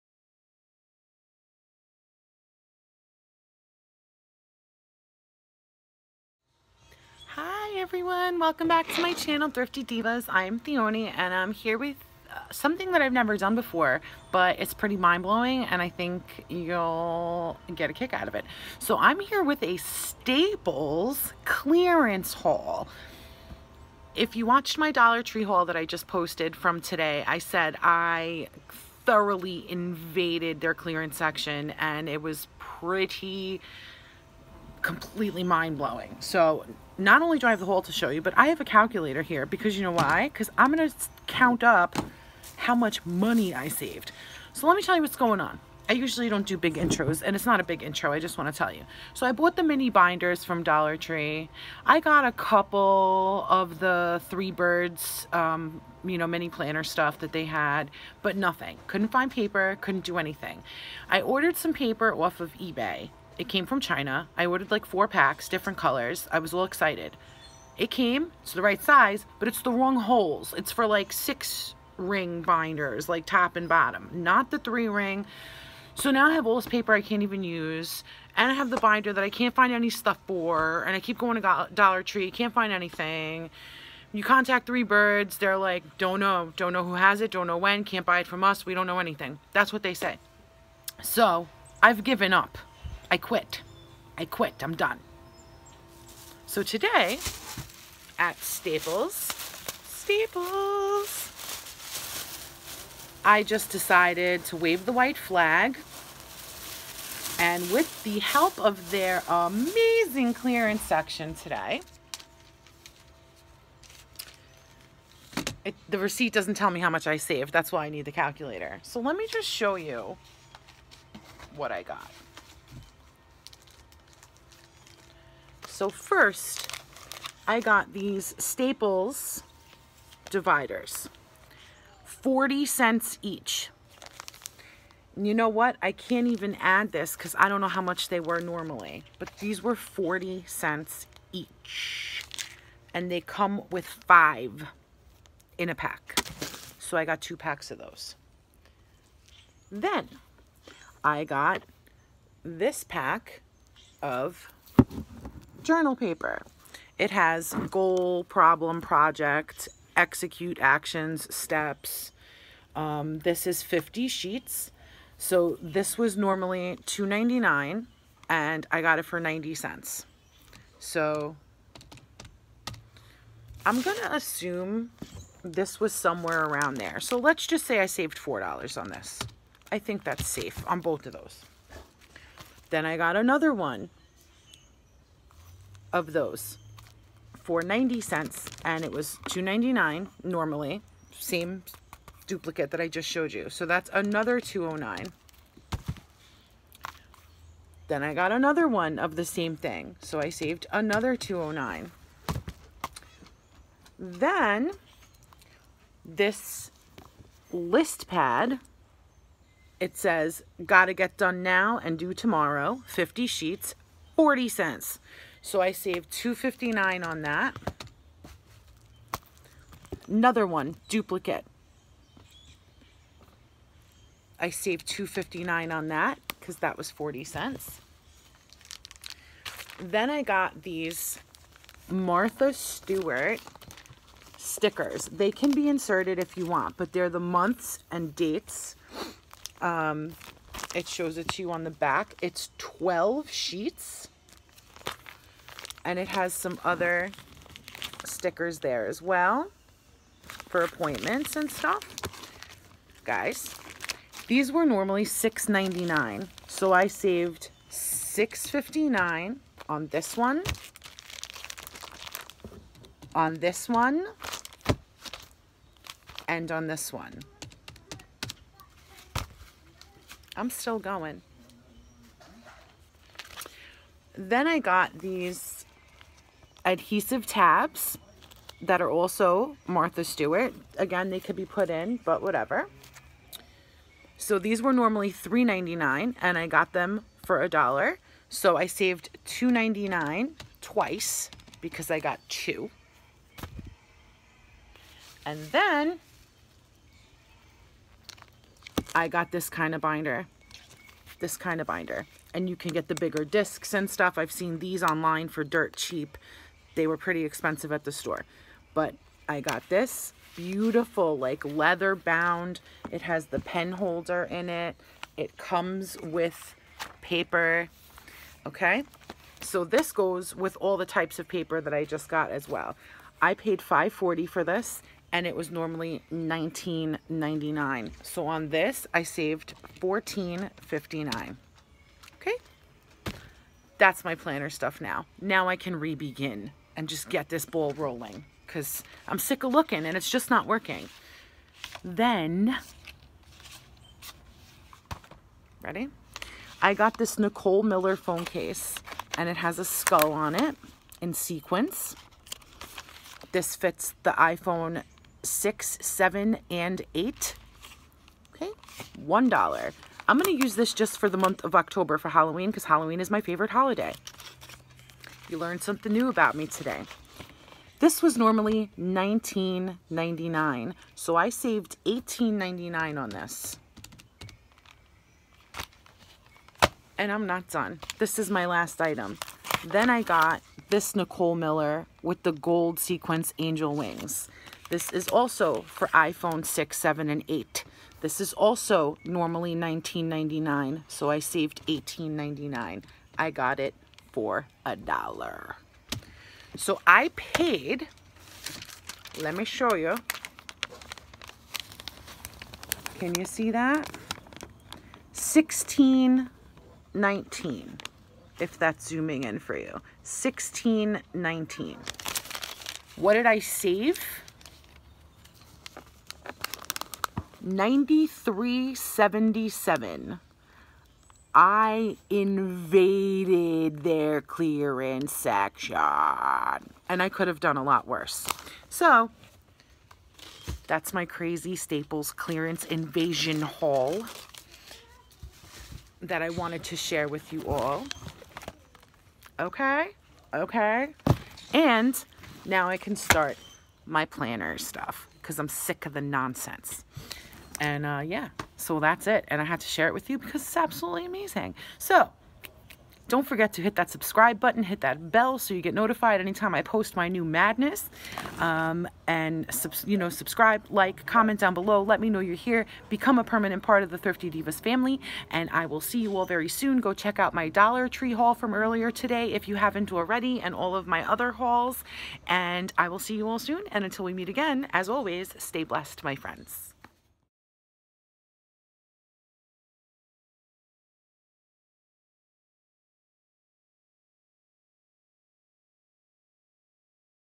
STAPLES INSANE CLeARaNCE HAUL!!!!!🙊 4/13/19
I saved almost A HUNDRED DOLLARS in the Staples clearance section today! iNSANITY! 4/13/19
Come see what I got!